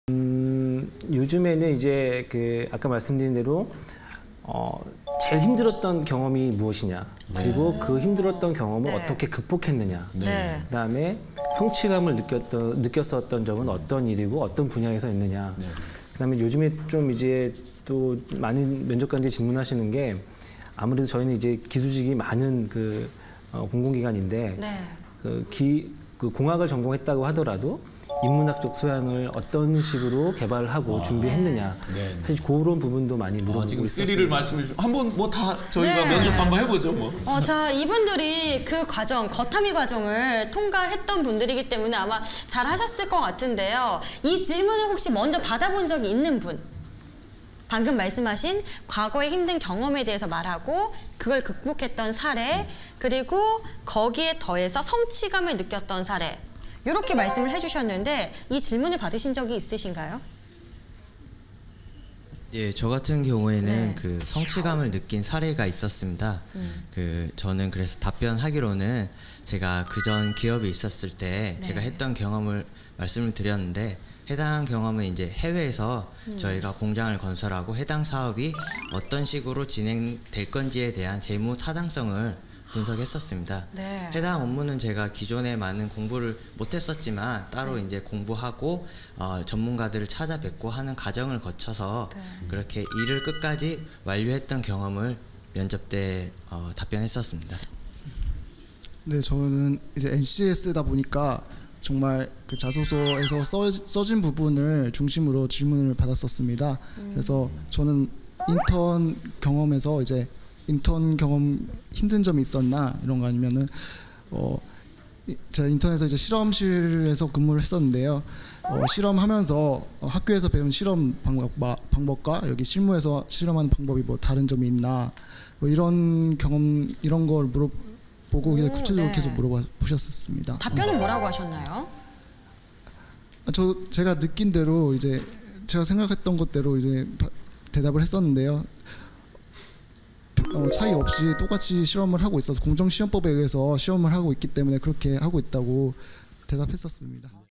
공기업, 제일 잘나오는 면접 질문 BEST3! 그리고 합격 답변_한국환경공단편
이것만은 꼭 대비하고 가야하는 면접질문,
나의 답변과 합격자들의답변을 비교해보세요!
면접준비의 제일 첫번째가 되어줄 거에요!!